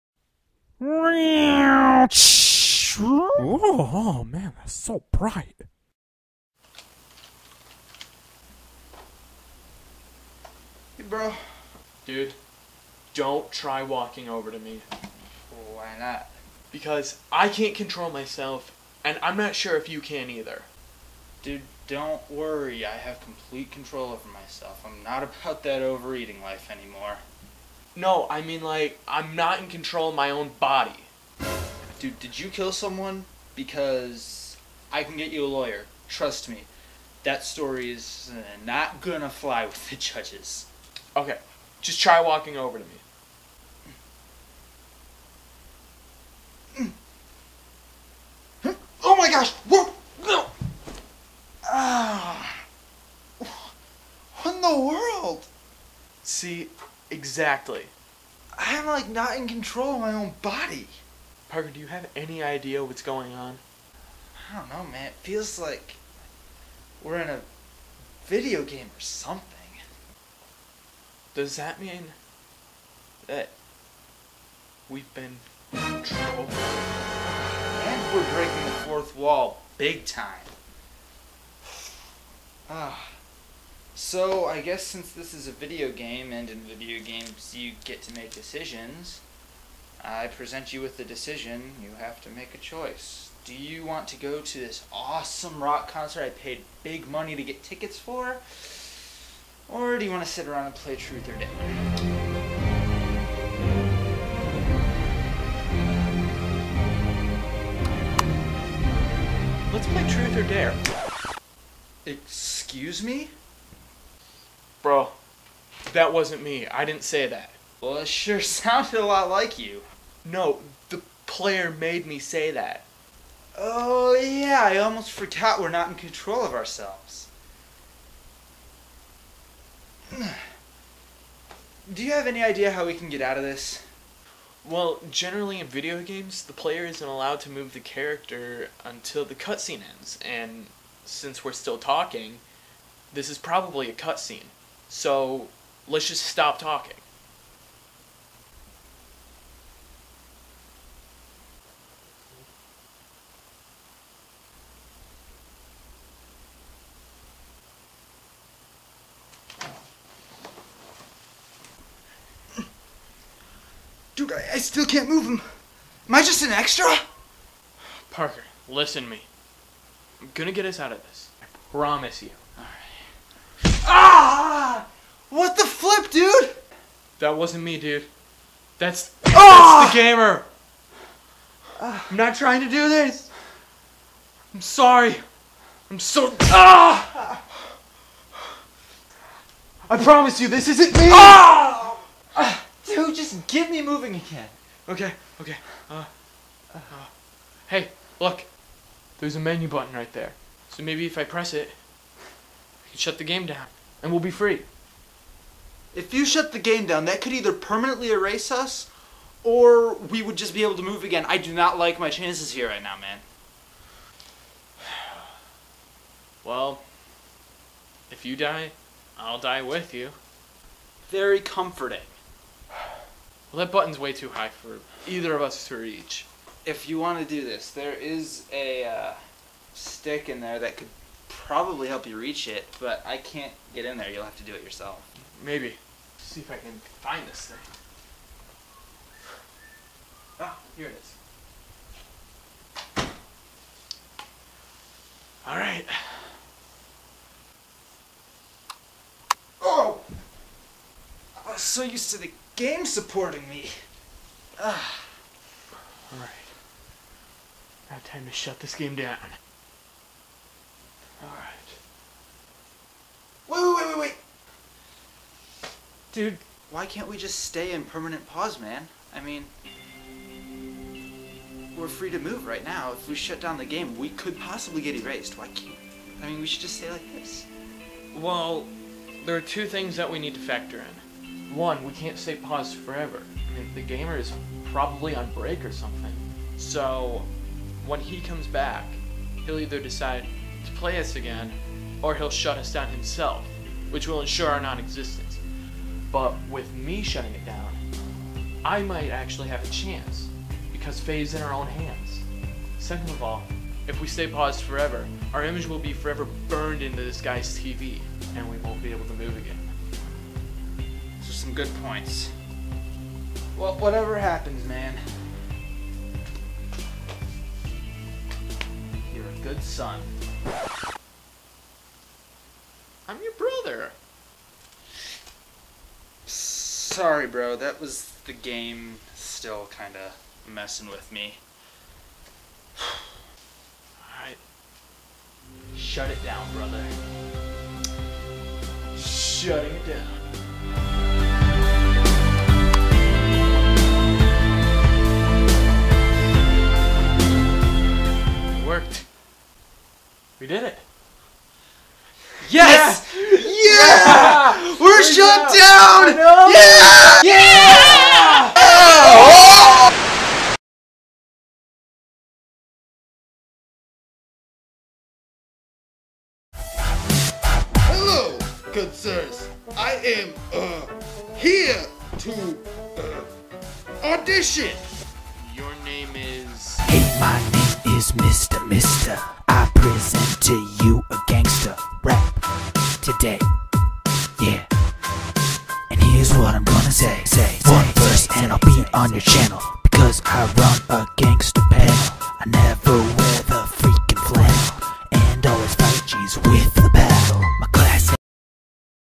LIFE AS A VIDEO GAME
Check our latest skit: "WALKING: That Awkward Moment When... (SHORT SKIT)"

Parker and Connor are under control of a gamer. How will they break free?